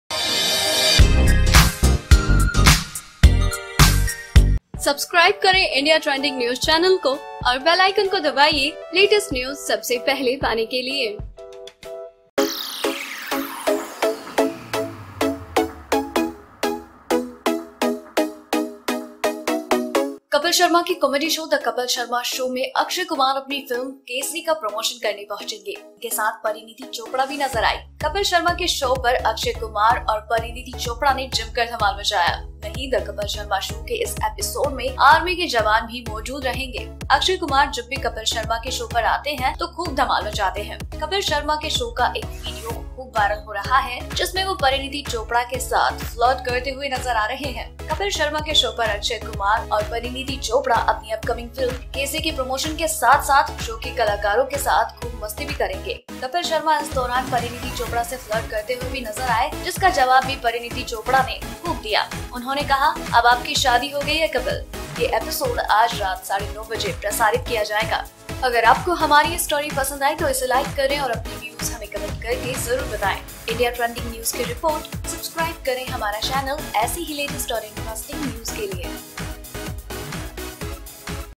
Kapil Sharma Flirts With Parineeti Chopra | कपिल शर्मा ने परिणीती के साथ किया फ़्लर्ट !!
India Trending News (ITN) : Kapil Sharma Flirts With Parineeti Chopra.